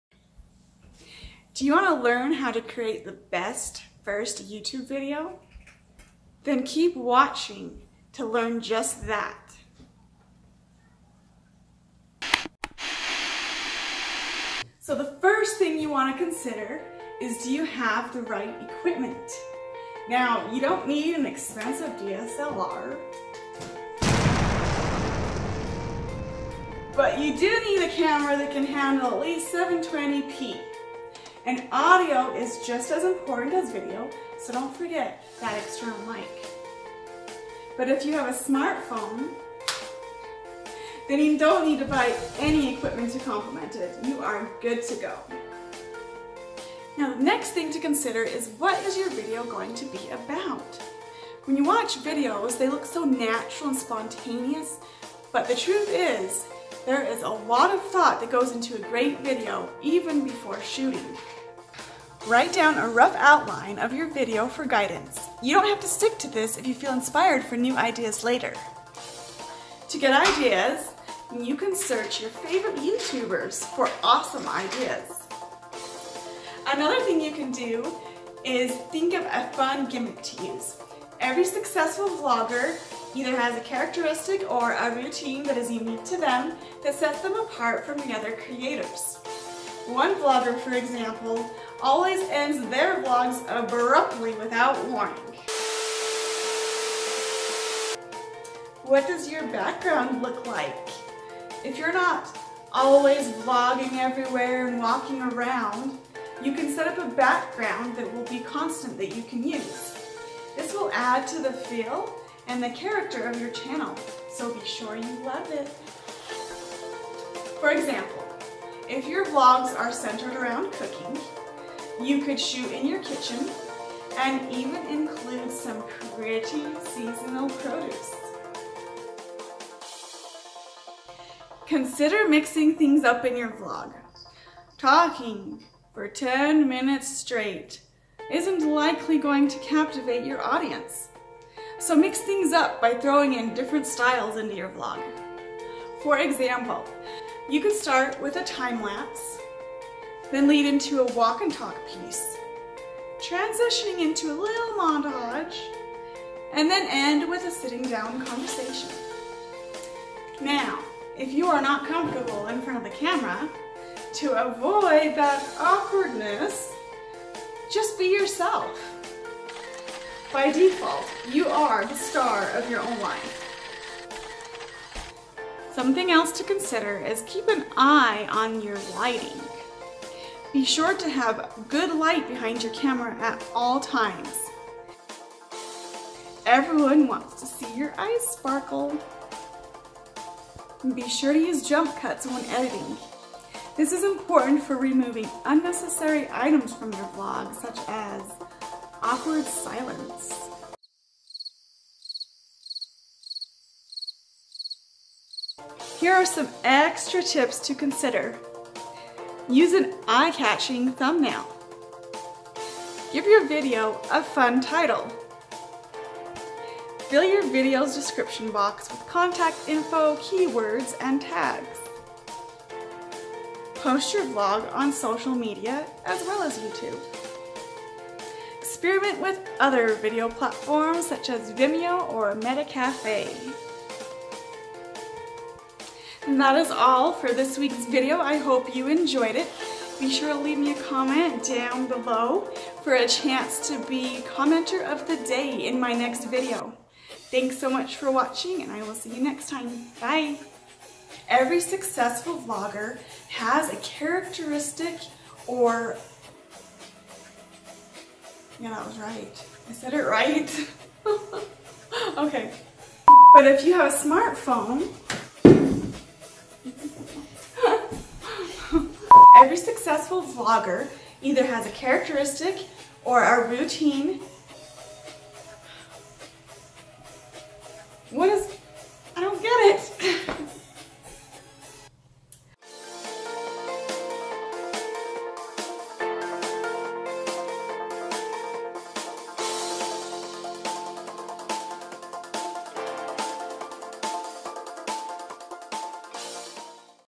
How to create an Epic vlog
How to create an epic vlog -- it's easier than you think!
This is the best YouTube video to show you how to create your first YouTube video- and it can be done with these few simple steps.

I've been creating vlogs for a couple years and I have learned a few simple tricks that I'm sharing to help you out.
From what equipment do I need to start a youtube channel to how to create a seamless video that will get views.

I hope you enjoy watching this as much as I enjoyed creating this video!

⭐If you are reading this, you are truly an awesome person!⭐
⭐Thanks for hanging out with me today!⭐